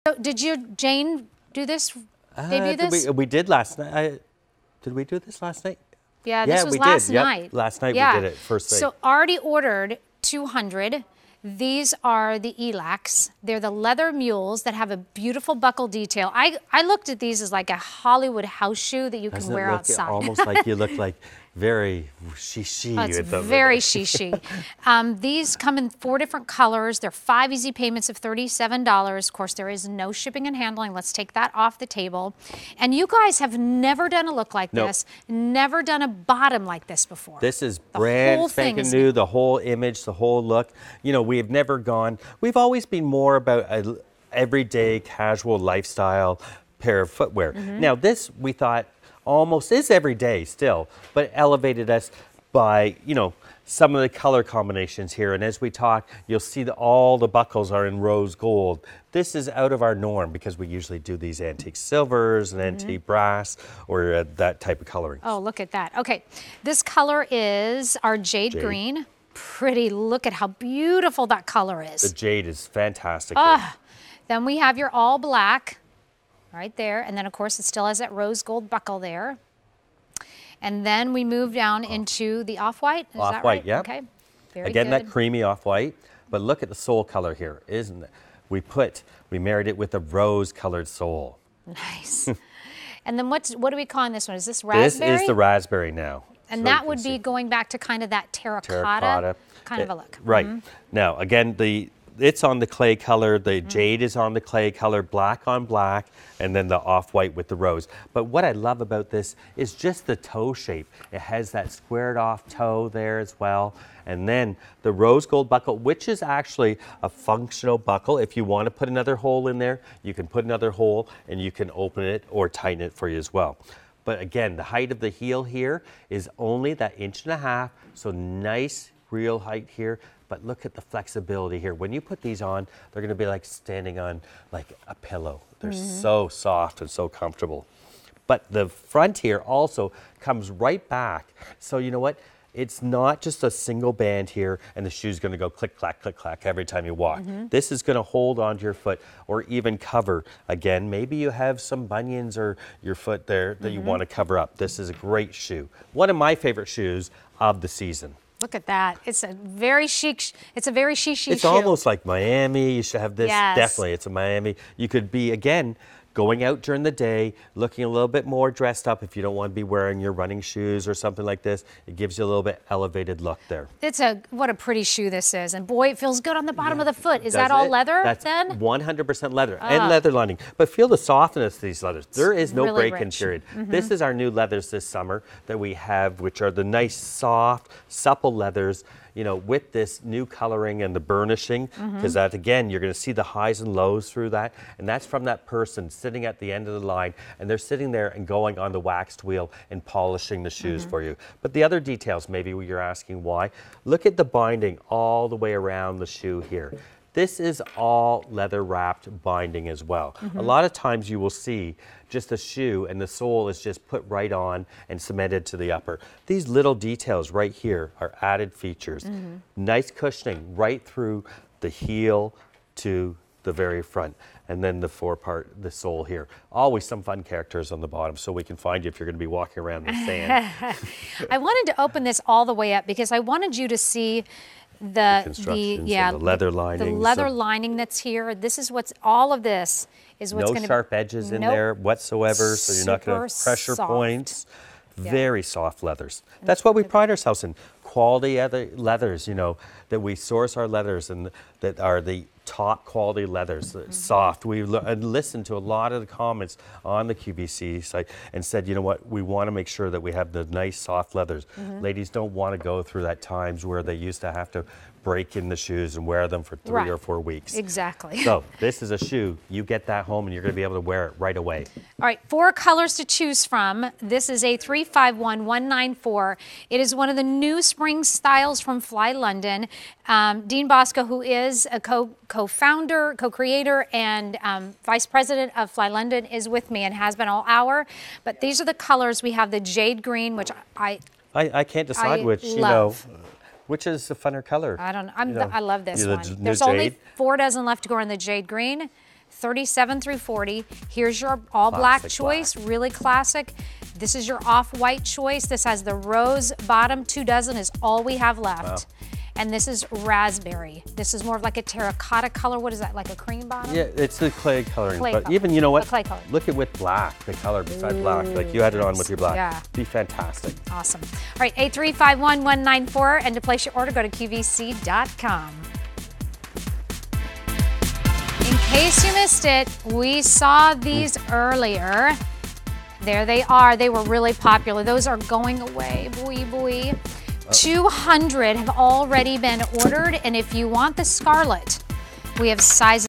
FLY London Leather Mules with Buckle Detail - Elax on QVC
FLY London Leather Mules with Buckle Detail - Elax

Relax and let these mules take the lead--whether you're aiming for sophisticated or modern, these shoes are your starting place for an incredible outfit. From FLY London.